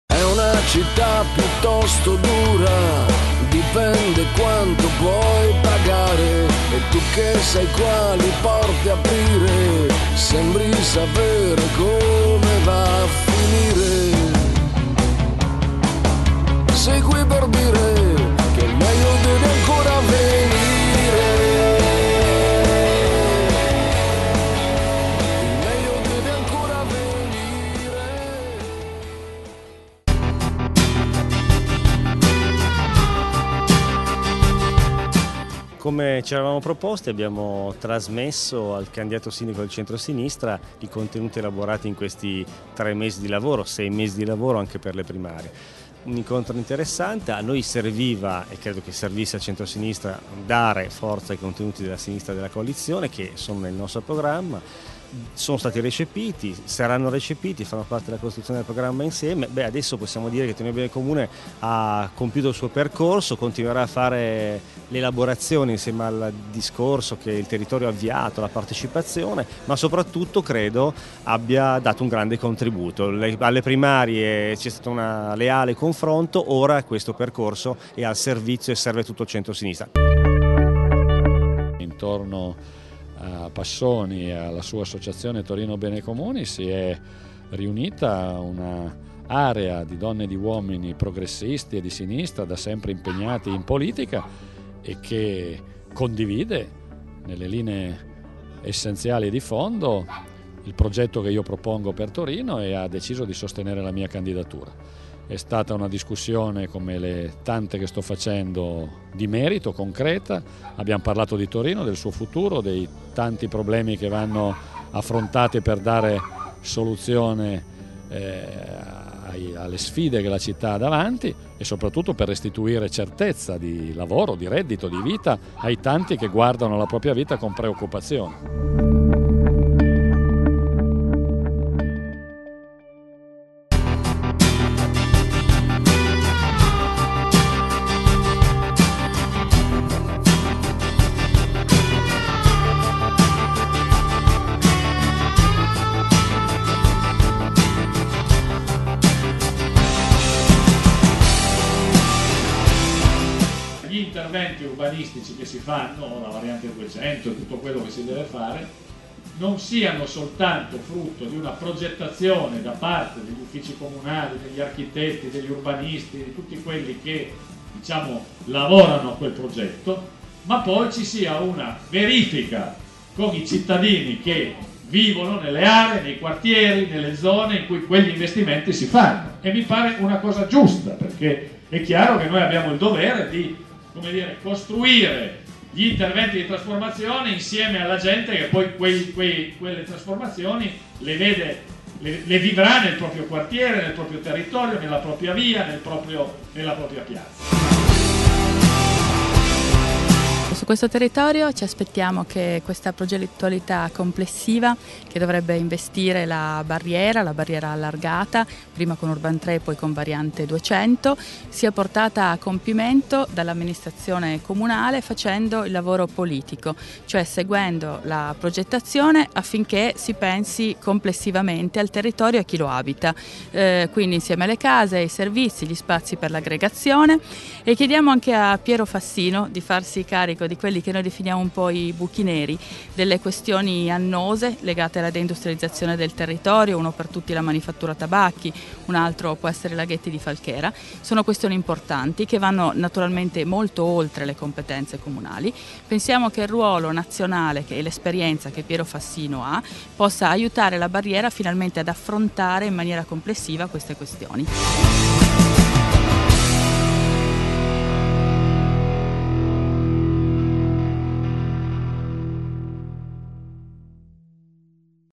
Diario di Piero - Incontro con la Circ.6 e Torino Bene Comune
La giornata di Piero Fassino raccontata attraverso i suoi incontri pubblici, le sue giornate nei quartieri della città, i dibattiti e gli appuntamenti tematici. Un programma da non perdere per seguire il percorso di Piero verso le elezioni del 15 e 16 maggio 2011.